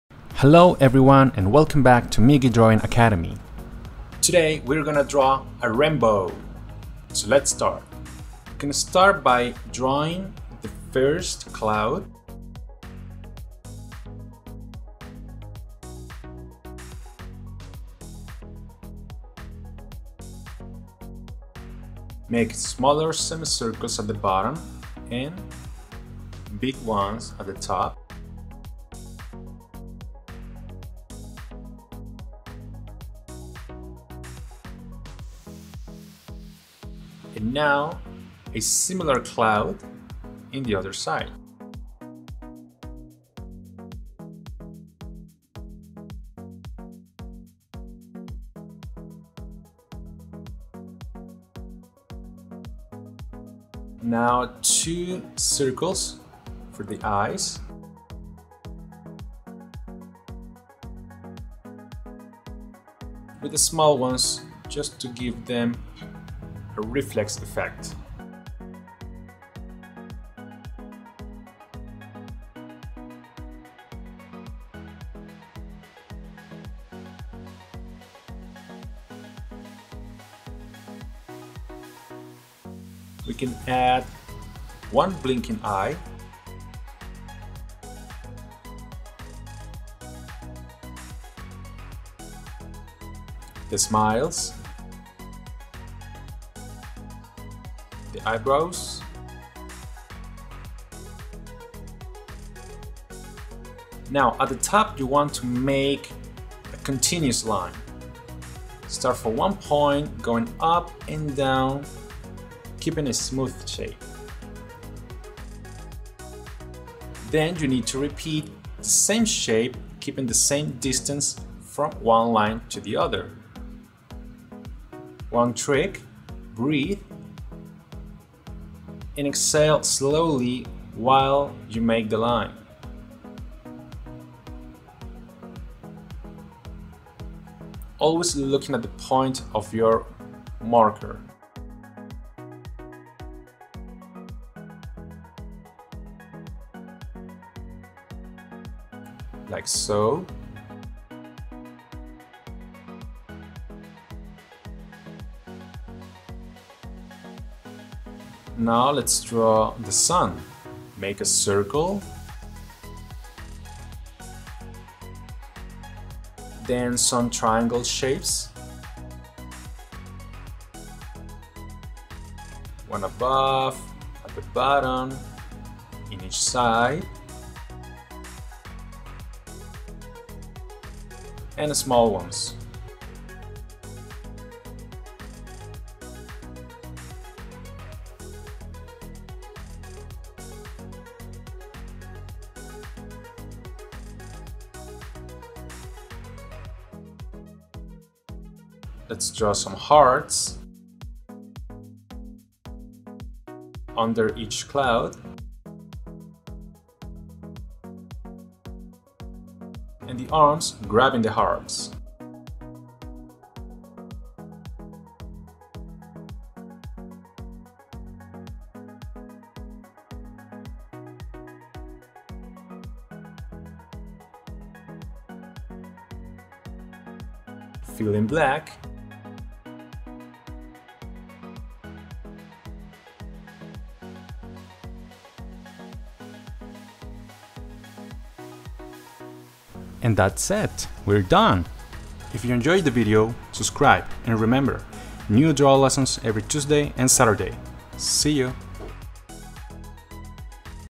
How to Draw a Rainbow EASY
Learn how to draw a simple Rainbow! with this tutorial step by step!